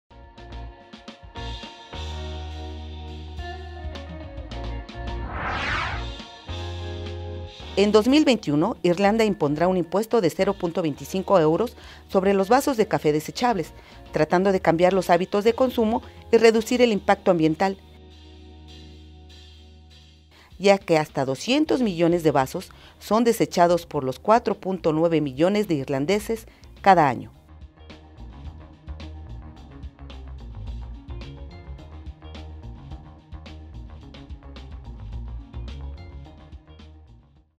Aplican impuesto a vasos de café, arranca en Irlanda
Irlanda impondrá un impuesto sobre los vasos de café desechables, en 2021 ,en un intento por reducir el impacto ambiental.